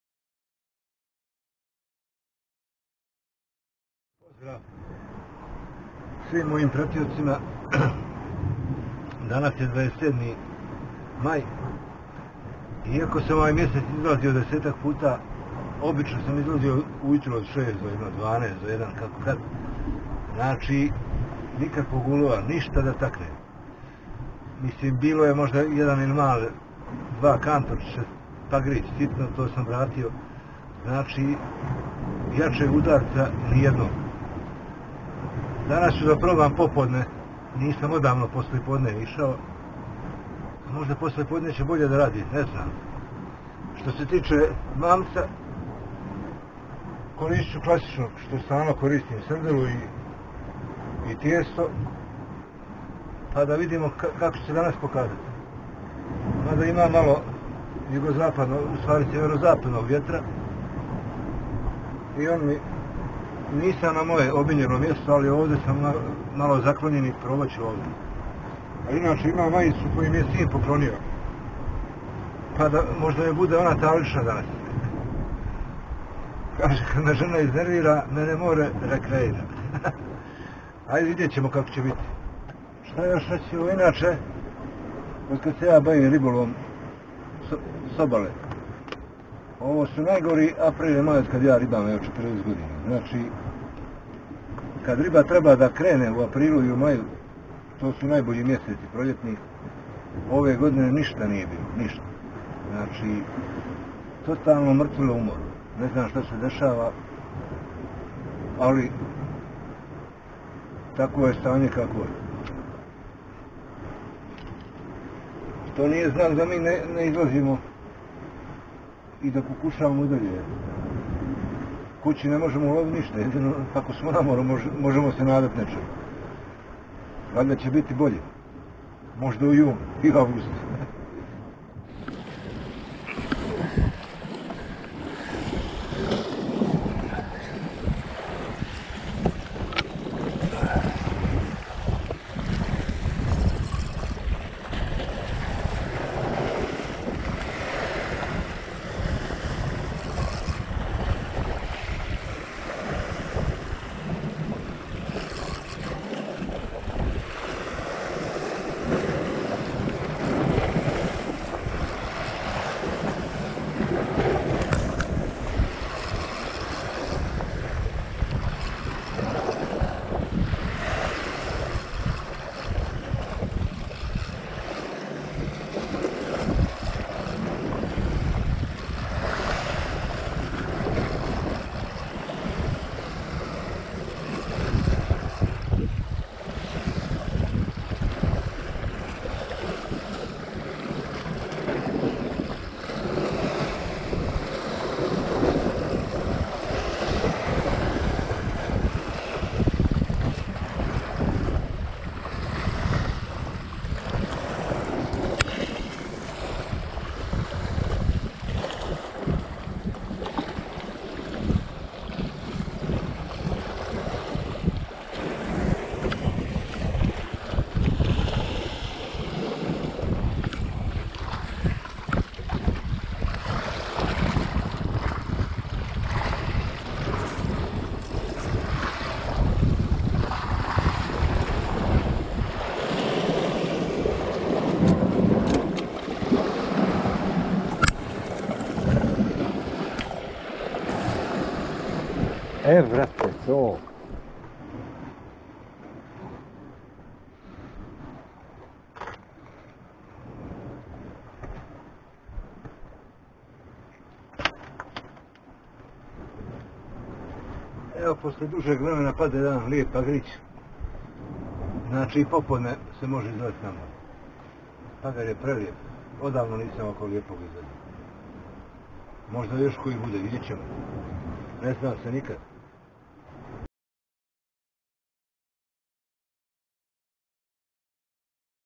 Ribolov štapom s obale-Pagar(1,1kg.) prvi majski ulov
Evo posle desetak izlazaka u ovom mjesecu da uhvatim napokon nešto da valja uhvatih lijepog Pagra.Koristio sam kao mamac srdelu i lignju ali je uhvačen na lignju Štap koji sam koristio Shimano Vegance Monster 300 ak 28-75gr, mašinica Daiwa BG4000 najlon Carp extrem 0,35mm i udica Spro 2/0 a dubina na mjestu ribolova je oko 20m.Bavim se ovom vrstom ribolova preko 40 god. i ova dva protekla mjeseca su najgora što se tiče ulova do sada.